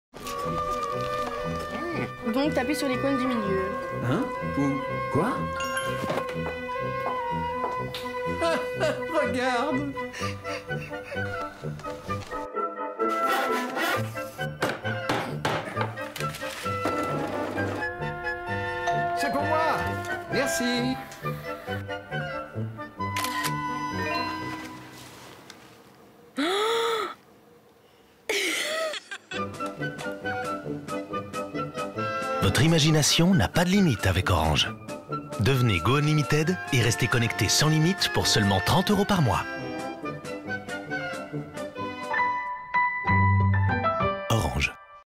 Abonnements - Go Unlimited SE - votre imagination n'a pas de limite
NOTICE : ceci est une vidéo qui est publiée pour des raisons éducatives. Nous ne garantissons plus l'exactitude de son contenu. Toutes les infos sur nos produits sont
Période : 12/2020
Message : devenez Go Unlimited et restez connectés sans limites pour seulement 30 euros par mois.
Média : TV